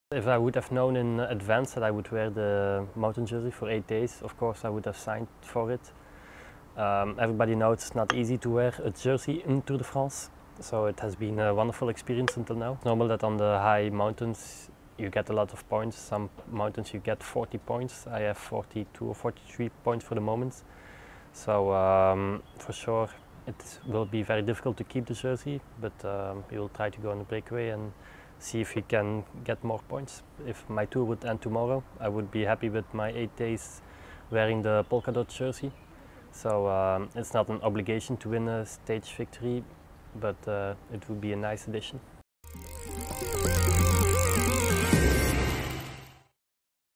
Wellens happy with Tour so far
Lotto Soudal's Tim Wellens is delighted to be in the polka dot jersey at the Tour de France but has more targets to achieve.